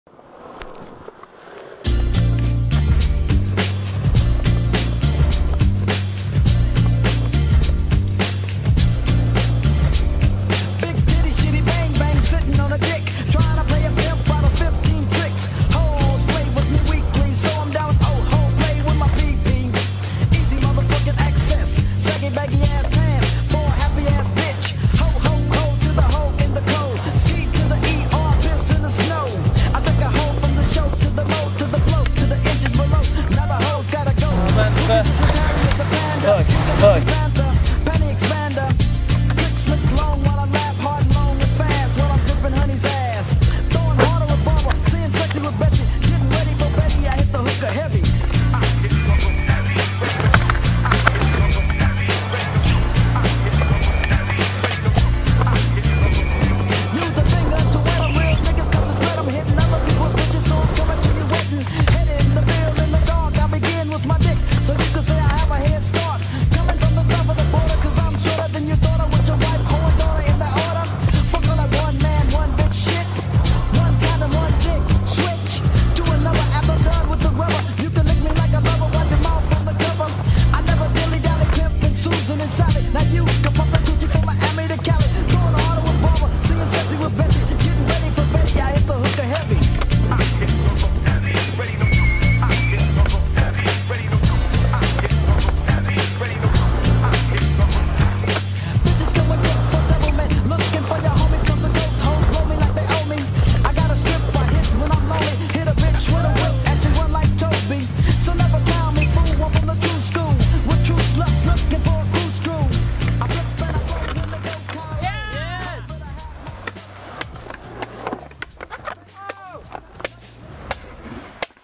Sketchy of the day #1 Faenza
First time in Faenza's Skate plaza
In Order of Appearance : Alberto giacinti,Andrea Giacinti,Roberto Tini,Steven Cattan.